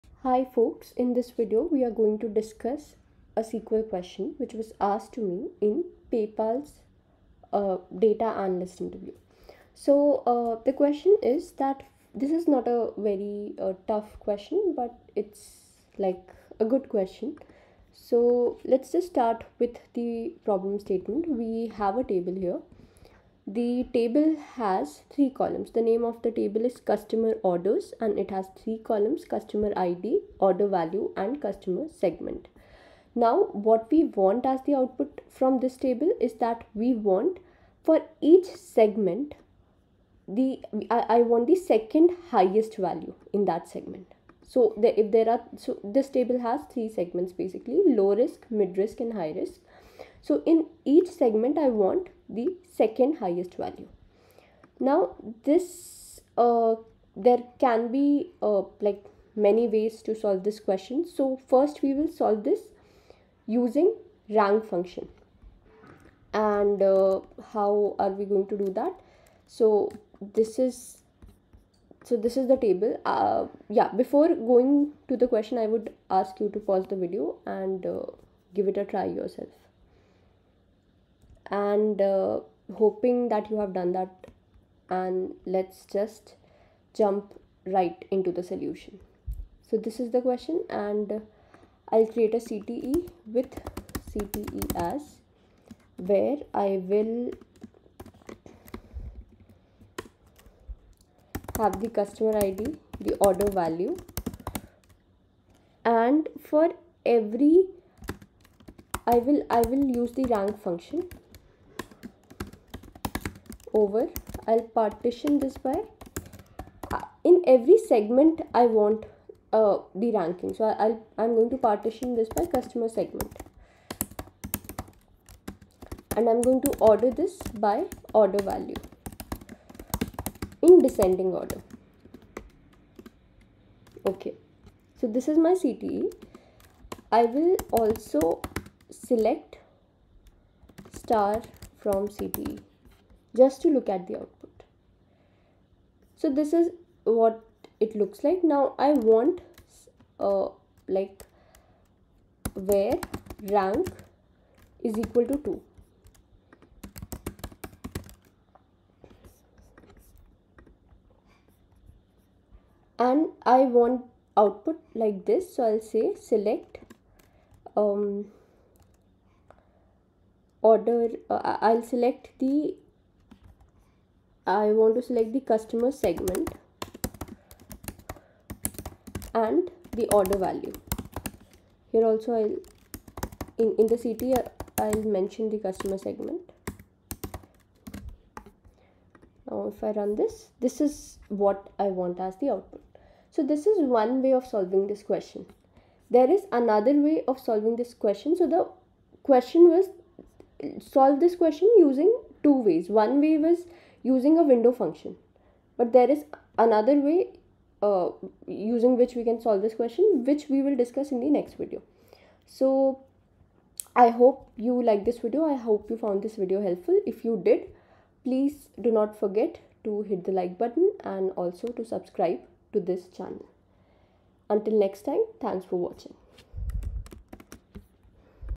Paypal Data Analyst Interview(part 1)- SQL Query
A question very similar to this was asked to me in one of the  rounds of interviews at Paypal.